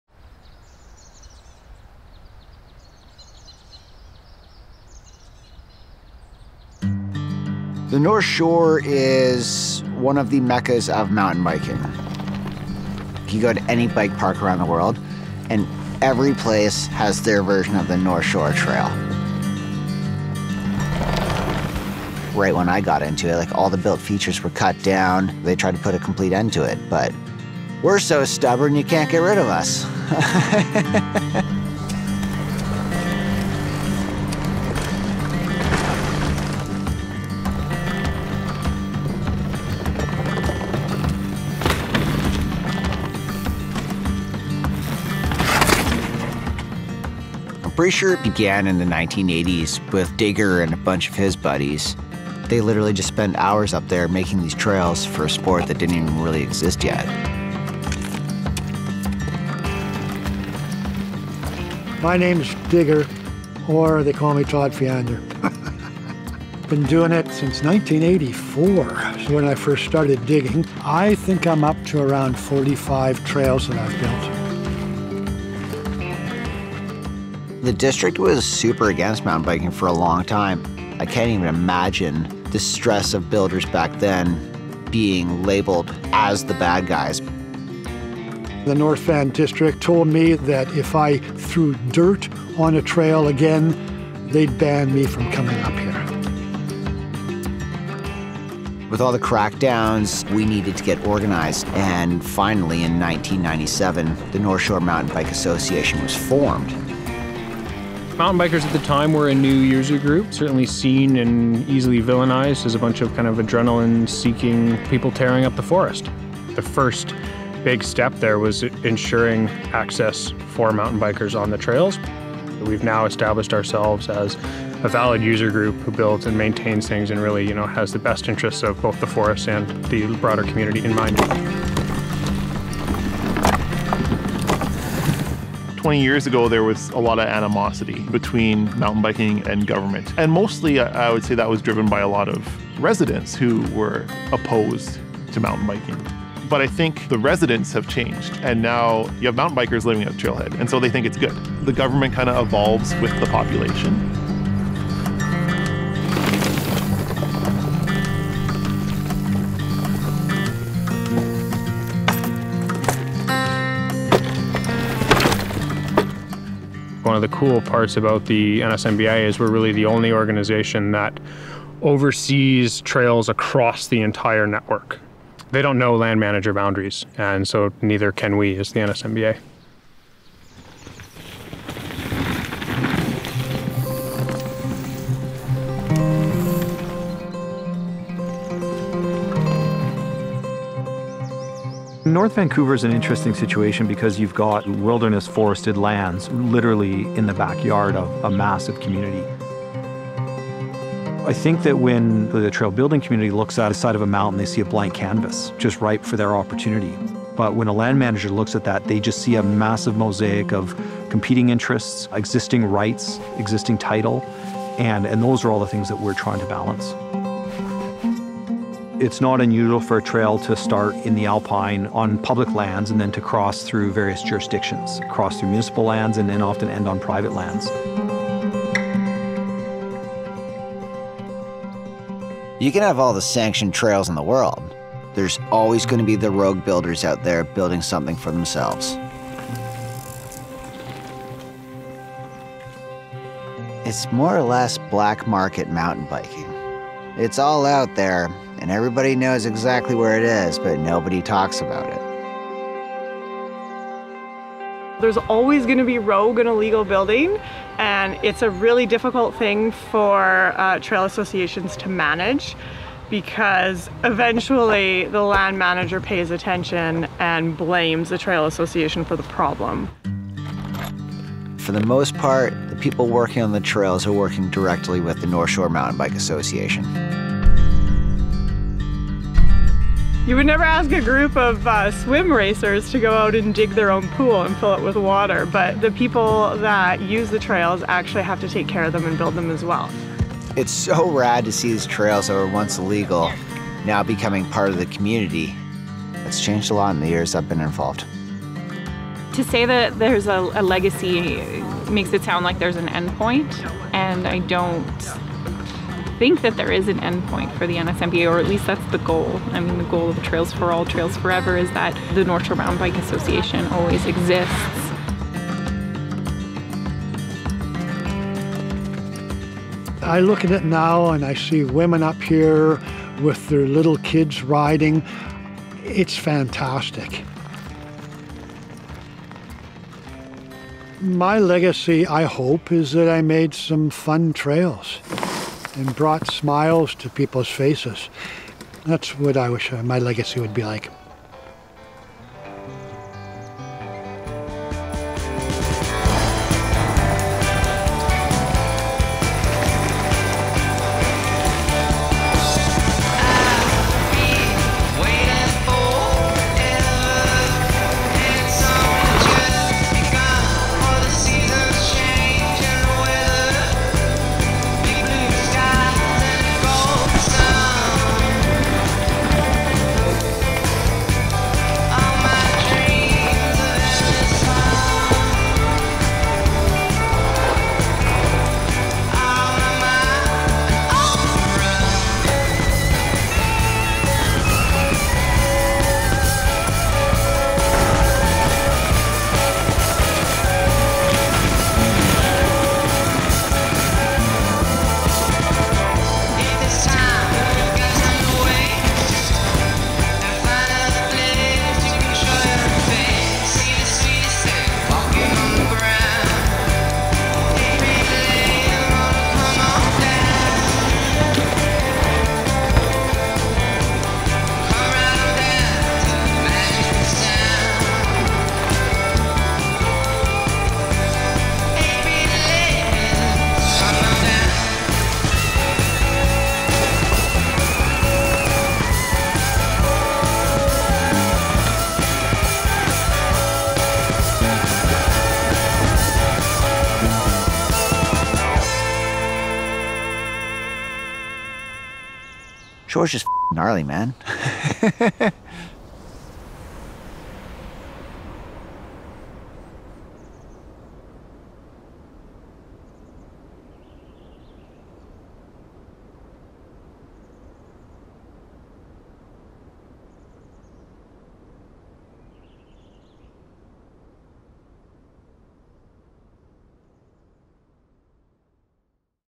The Outlaw Sport | TRAIL TALES
The history and legacy of trail building in Vancouver’s North Shore mountains is incredible, few places on earth have had as much impact on the mountain biking community as these Canadian mountains. Follow Geoff Gulevich and legendary trail builder Todd “Digger” Fiander on their quest to sanction mountain biking trails with the local government. With the evolution of mountain biking as a sport, came the due diligence of preserving the land for future generations to enjoy.